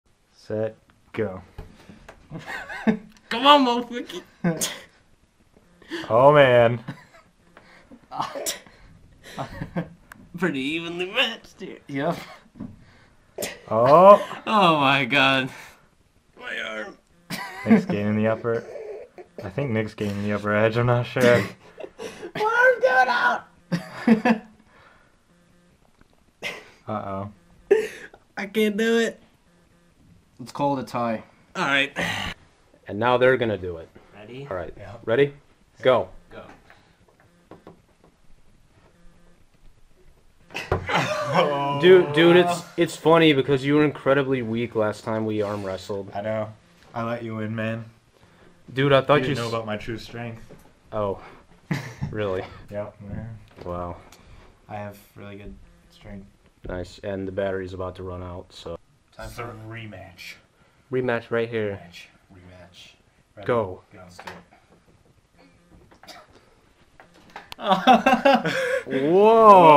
Armwrestling (124/365)
Dan vs Nick
Nick vs Karsen
Karsen vs Dan
IT'S ON!